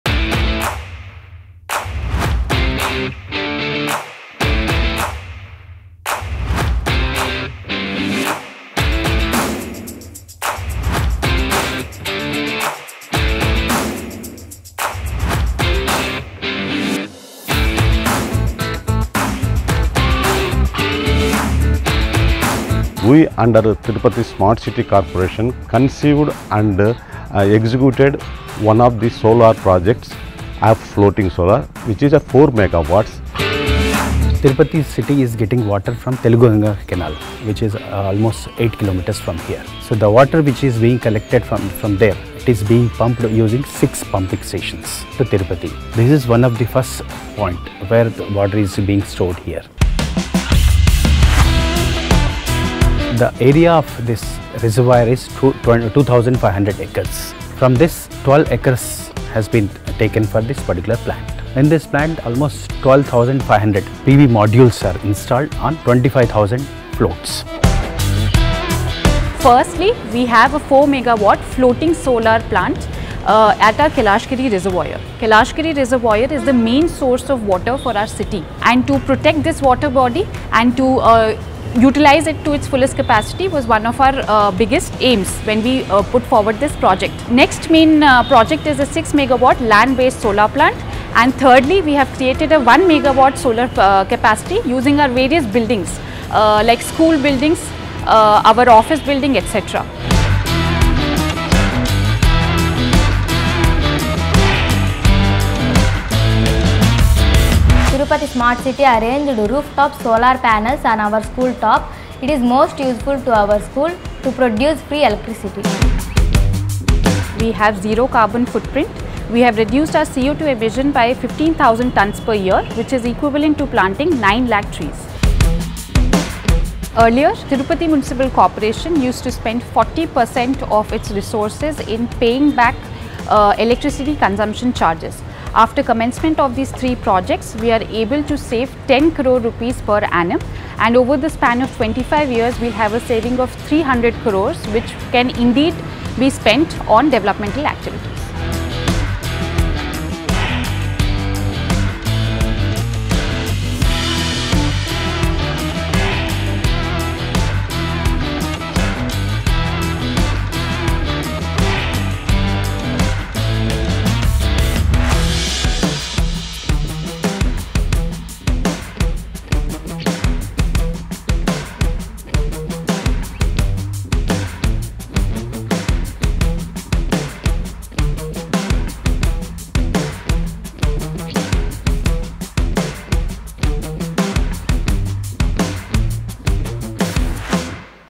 Pathways to a Sustainable Future Tirupati
Climate change is global reality. Watch the efforts of smartcitiesmission giving a clear direction to the progression of city actions to combat climate change. It’s the pathway to a sustainable future.